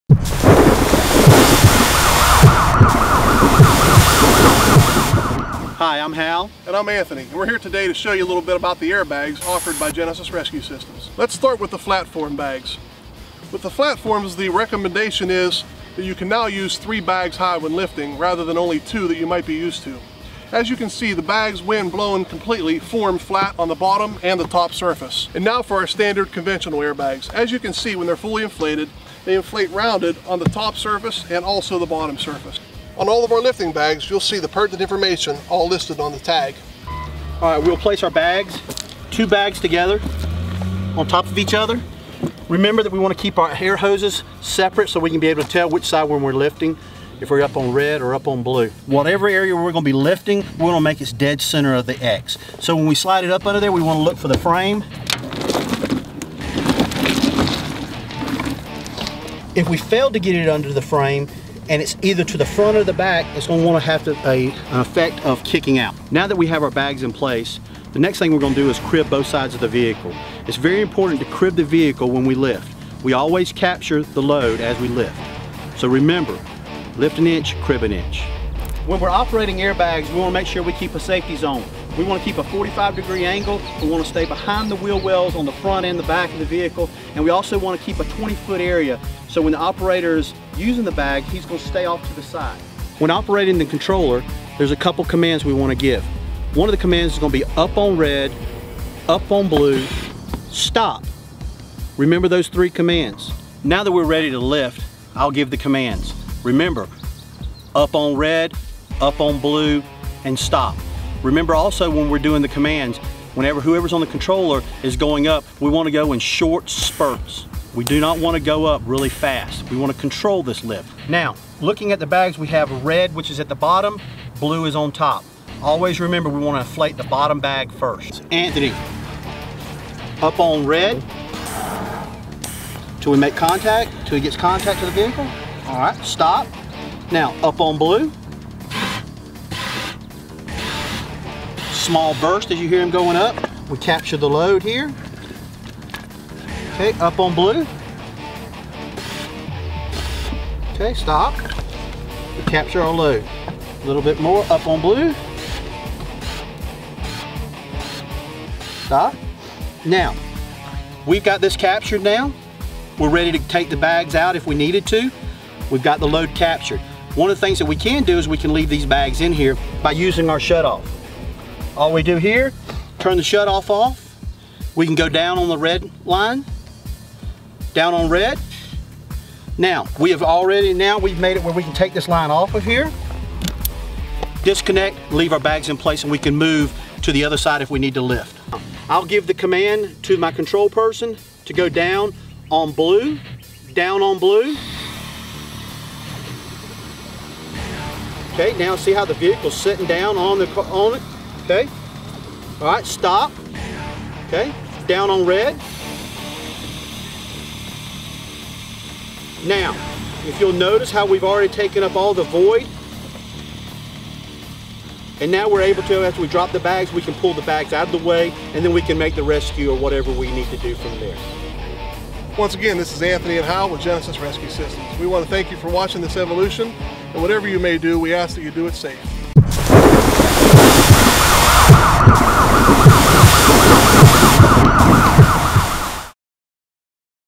High Pressure Airbags | Vehicle Stabilization | Genesis Rescue
Genesis Rescue Systems High Pressure Airbags basic operation.